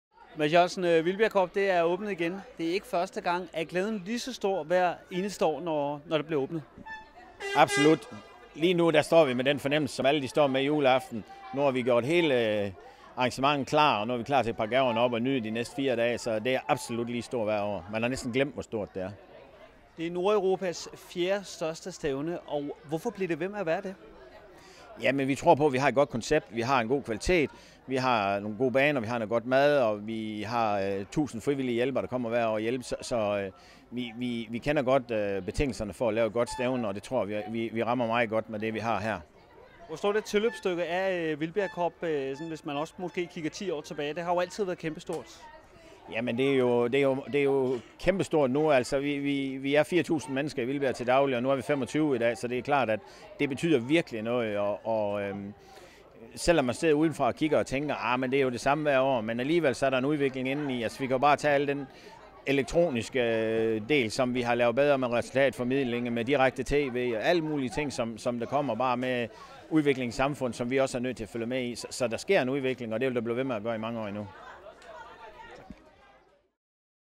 Mads er klar til Vildbjerg Cup 2019
Her et interview med formanden for Vildbjerg Cup.